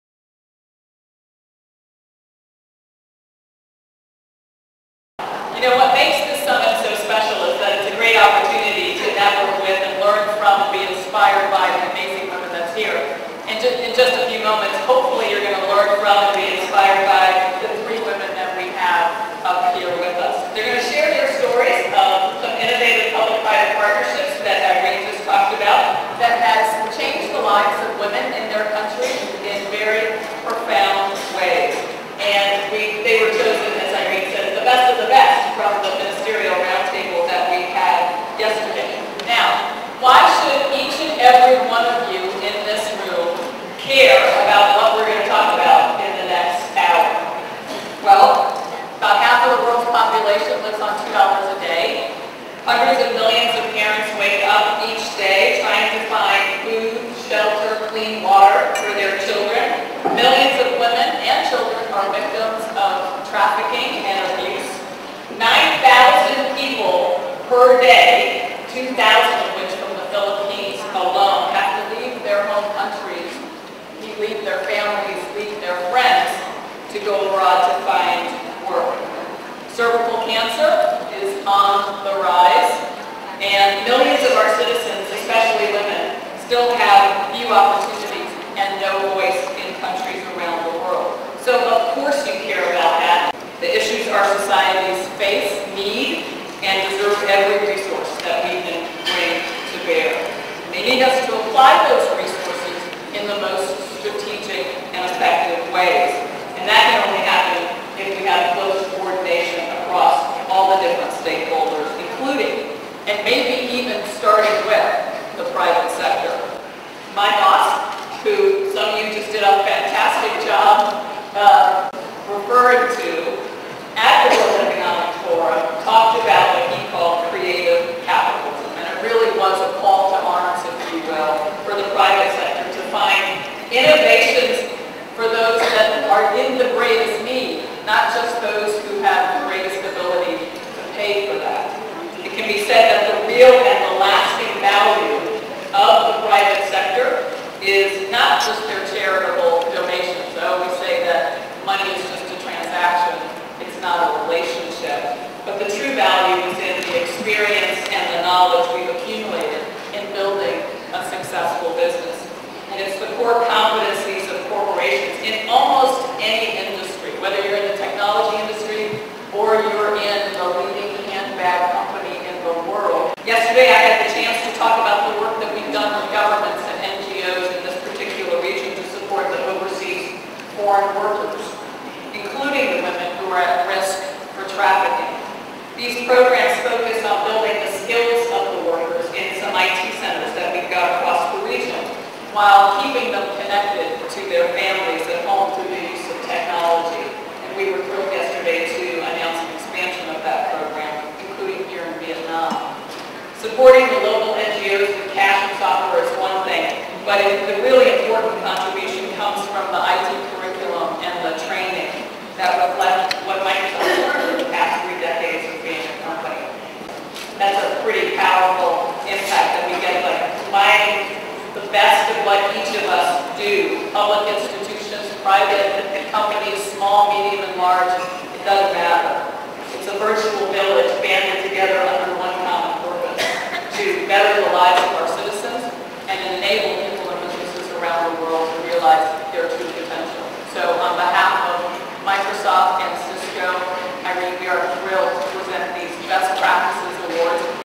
Global Summit of Women 2008 - Ministerial Rountable
Introduction to the "Report from the Ministerial Roundtable" by Gerri Elliot, Corporate Vice President for Public Sector, Microsoft